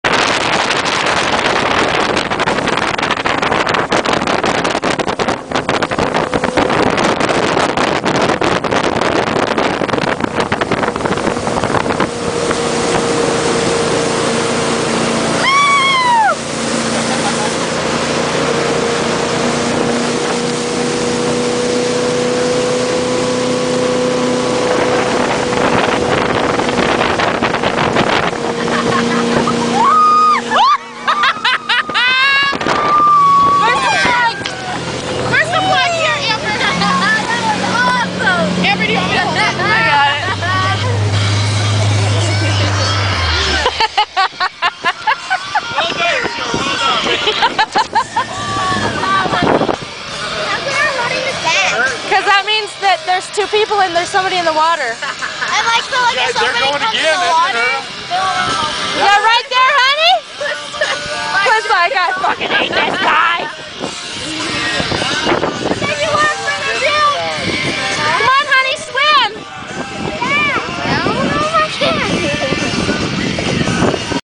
Rednecks on a raft!!
Clint and Mark wrecking on a raft